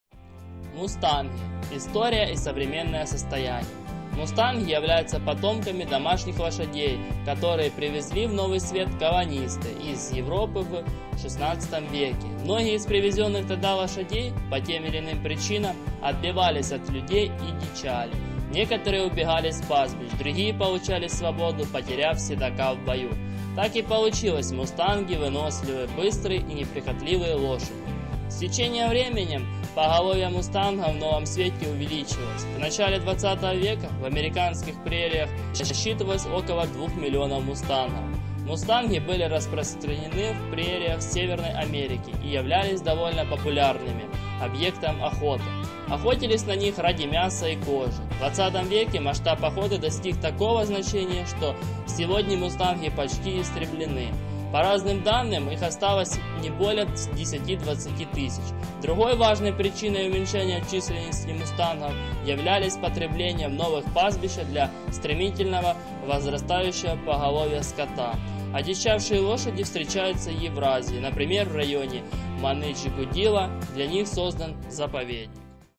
Порода лошади. Мустанг.Редкая лошадь,занесенная в заповедник.
Мустанги являются потомками домашних лошадей, которых привезли в Новый Свет колонисты из Европы в XVI веке. Многие из привезённых тогда лошадей, по тем или иным причинам, отбивались от людей и дичали: некоторые убегали с пастбищ, другие получали свободу, потеряв седока в бою. Так и получились мустанги, выносливые, быстрые и неприхотливые лошади.

В XIX веке одичавшие, полудикие лошади в большом количестве водились в Южной Америке, в частности, в Аргентине и Парагвае. Вот как описывались южноамериканские мустанги в «Энциклопедическом словаре Брокгауза и Ефрона»
С течением времени поголовье мустангов в Новом Свете увеличивалось: к началу XX века в американских прериях насчитывалось около 2 млн мустангов. Мустанги были распространены в прериях Северной Америки и являлись довольно популярным объектом охоты. Охотились на них ради мяса и кожи. В XX веке масштаб охоты достиг такого значения, что сегодня мустанги почти истреблены: по разным данным их осталось не более 10—20 тысяч. Другой важной причиной уменьшения численности мустангов являлась потребность в новых пастбищах для стремительно возрастающего поголовья скота.

Одичавшие лошади встречаются и в Евразии, например, в районе Маныч-Гудило для них создан заповедник